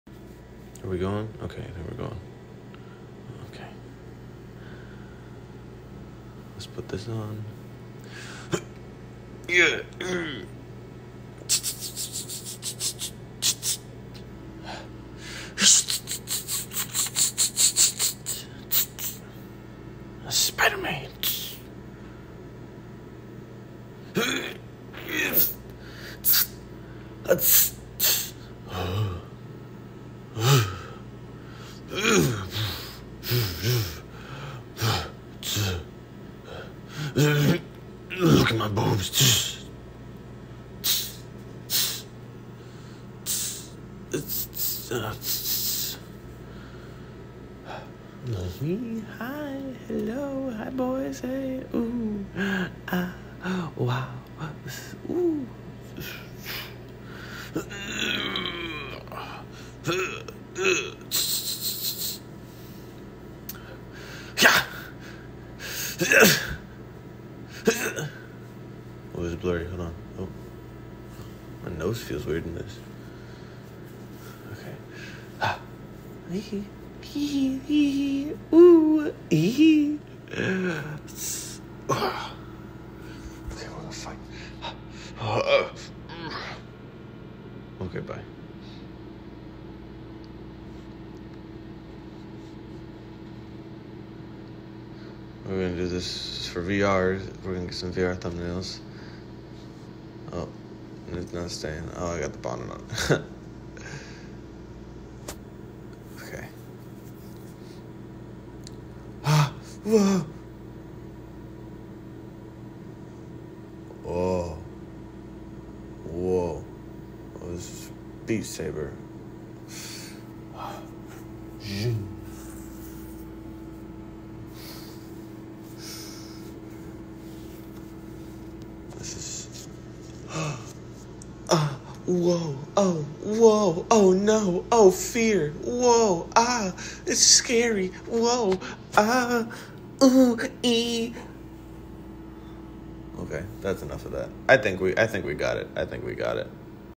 For Your Consideration: Spider-Maid!
Ever wanted to have access to some weird images of Spider-Man in a maid outfit? Maybe doing VR? Go ahead. I'm not here to ask why.